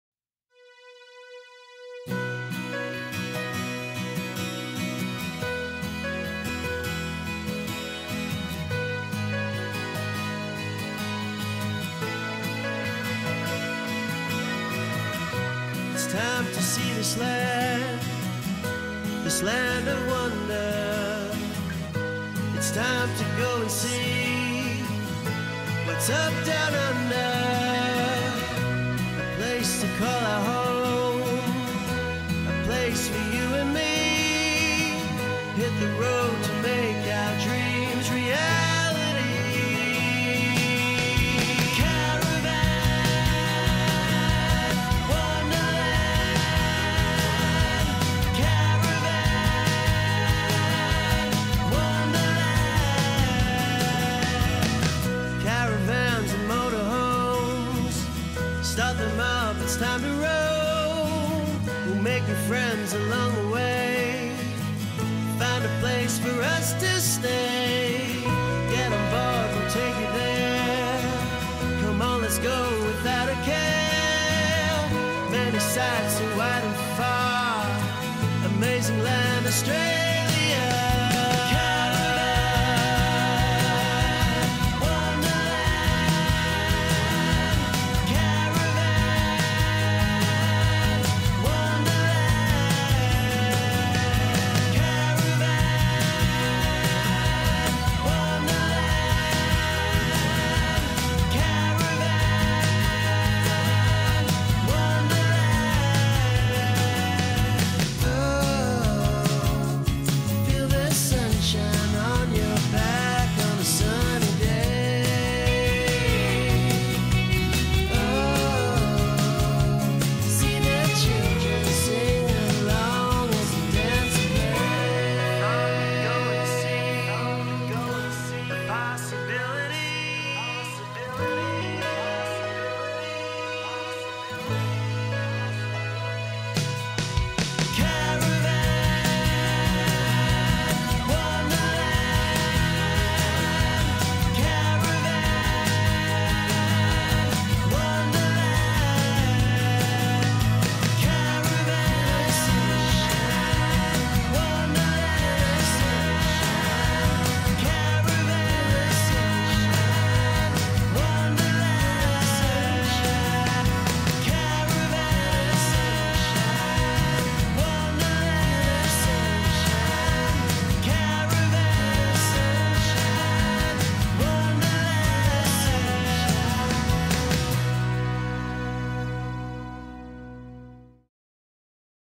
What's Up Downunder Convoy for Charity - New Age Manta Ray
In Oct / Nov 2013, the What's Up Downunder crew embarked on the second annual Go Make Some Memories Convoy for Charity.

The New Age Manta Ray featured in the convoy as one of the best of the best caravans & RVs of 2013.

Check out the hightlights!